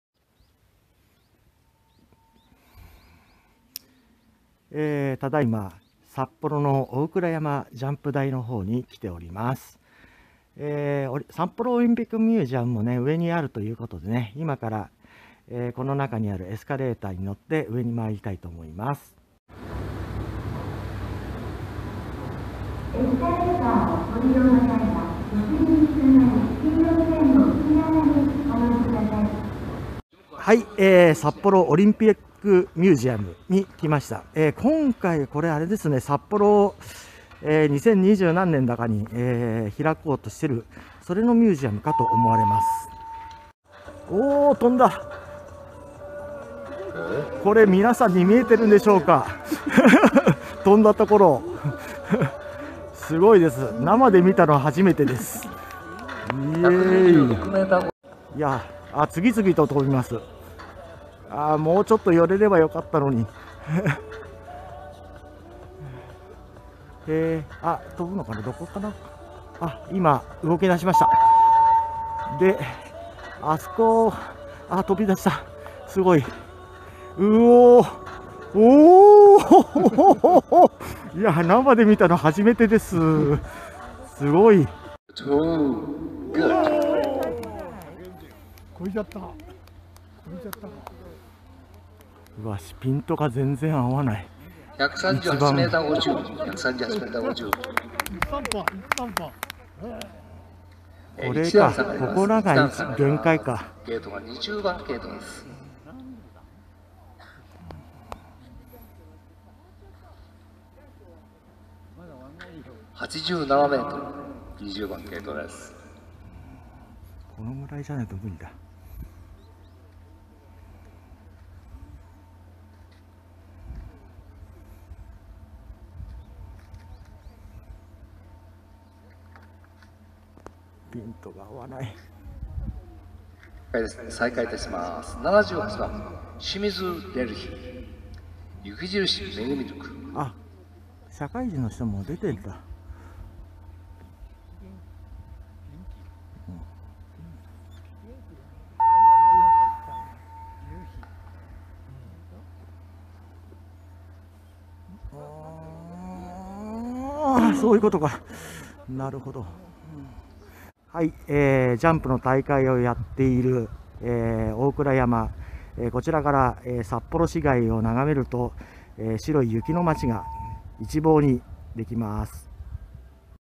冬の大倉山に行ってみた
真冬には珍しい快晴となった札幌市内。大倉山ジャンプ競技場を訪れたらジャンプ競技の大会が開催されていました。